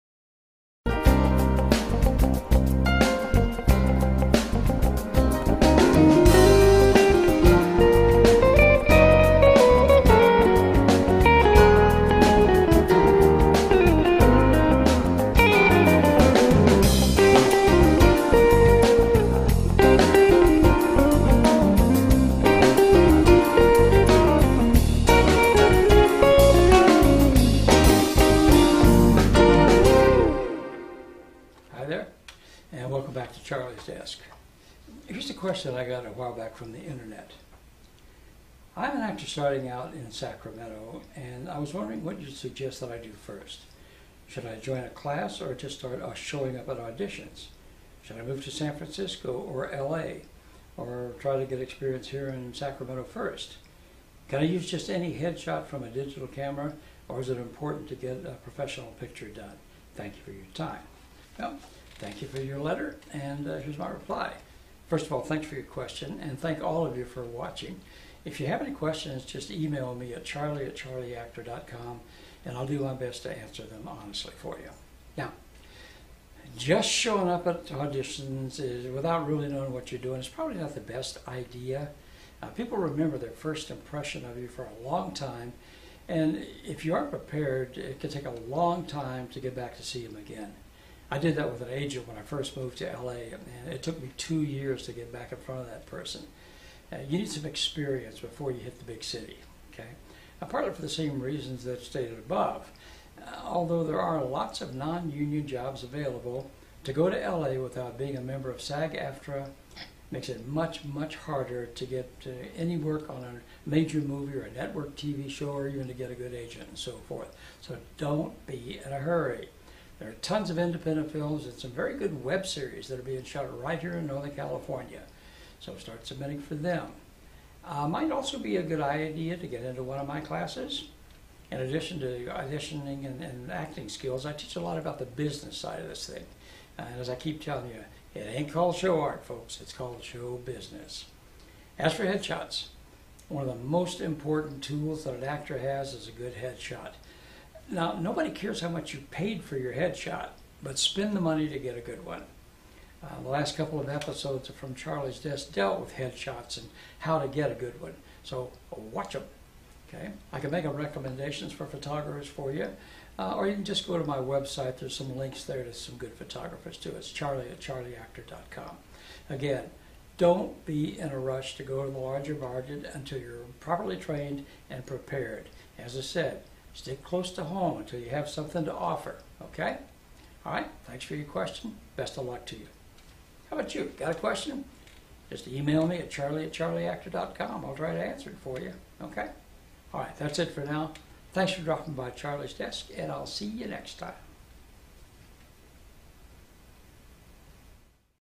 From Charlie's Desk - FCD 16 How Do I Start?
Tips from Charlie Holliday on the Business of Acting